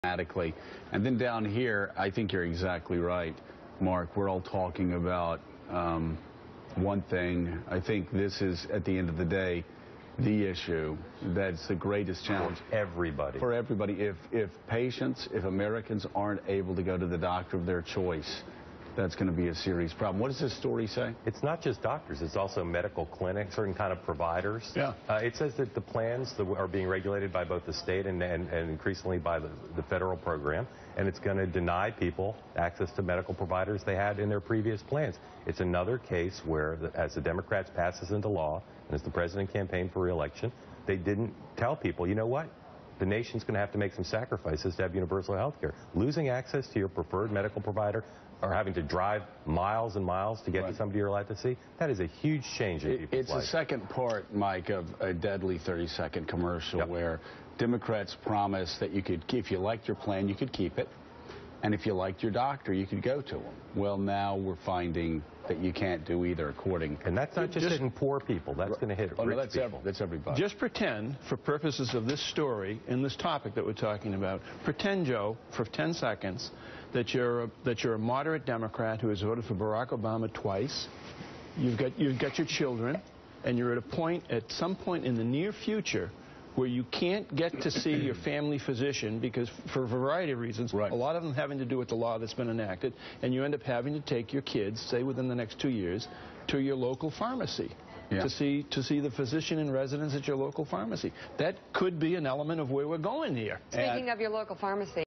MSNBC: ObamaCare Forcing Americans To Lose Their Doctors Will Be A Problem For Democrats
MSNBC: ObamaCare Forcing Americans To Lose Their Doctors Will Be A Problem For Democrats (February 6, 2014)